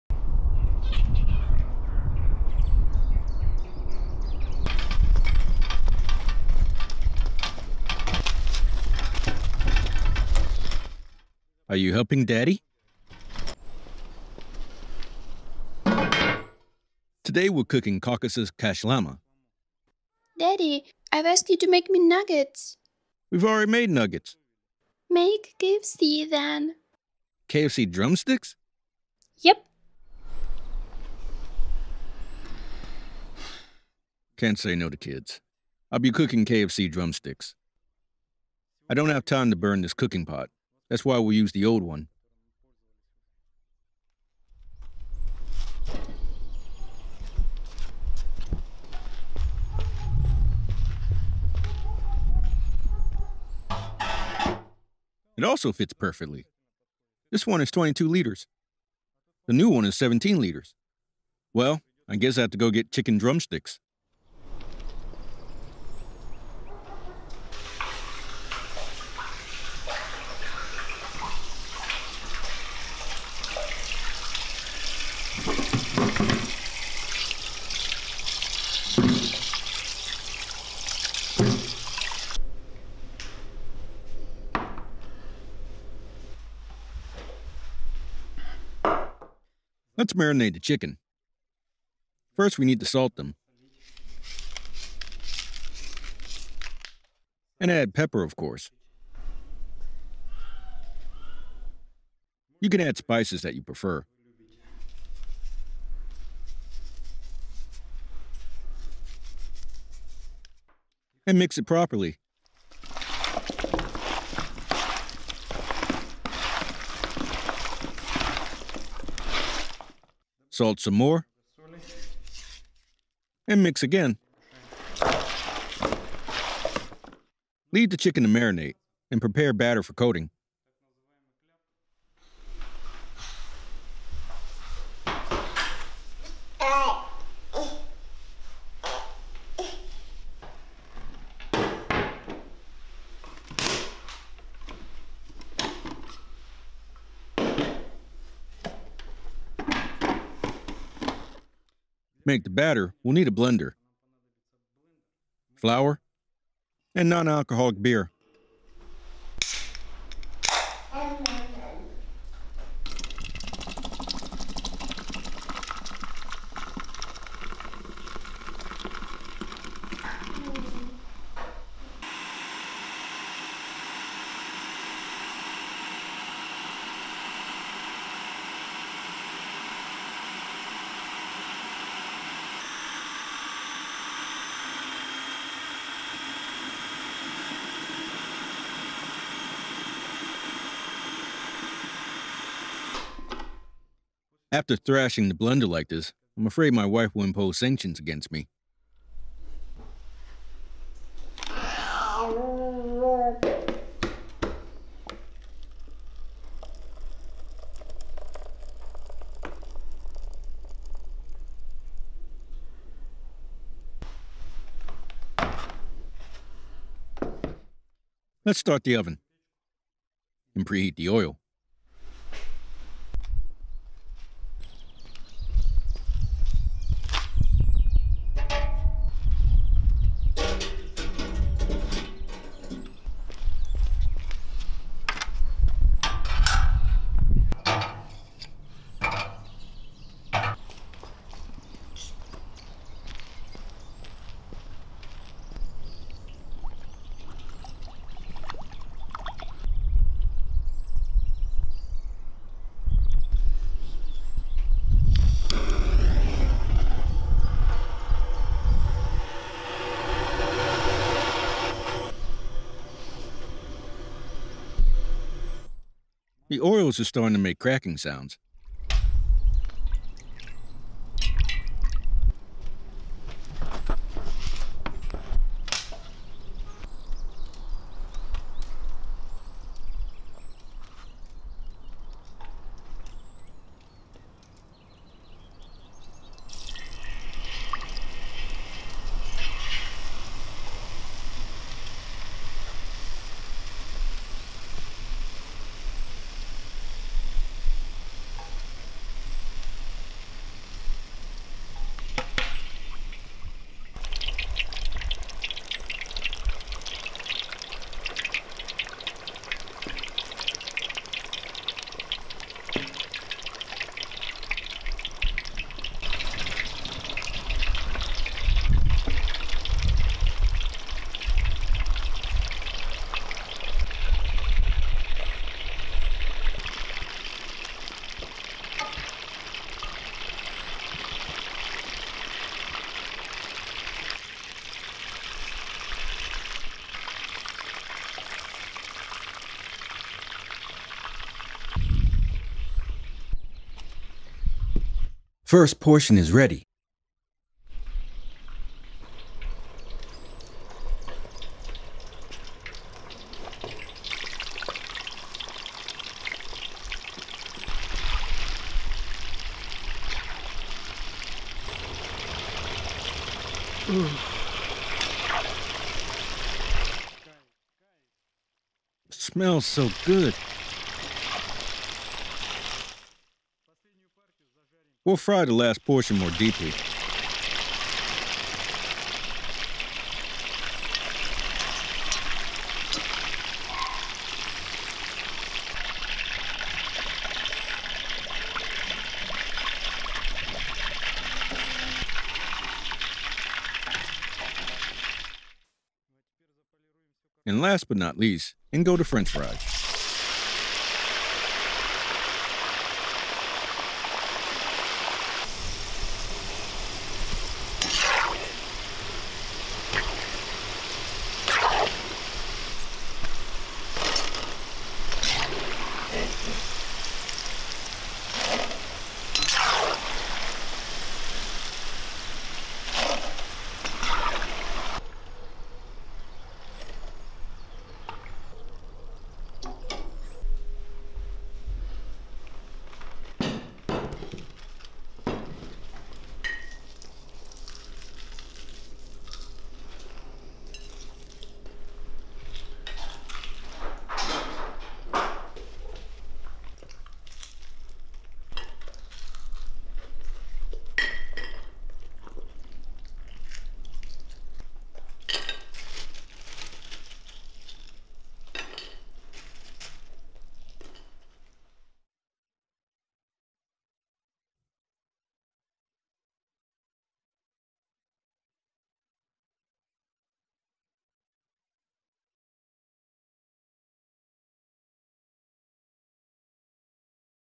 Fried chicken legs and french fries | GEORGY KAVKAZ
How to cook fried chicken legs and french fries in a cauldron. Best recipe for chicken.

If you have any questions, leave it below!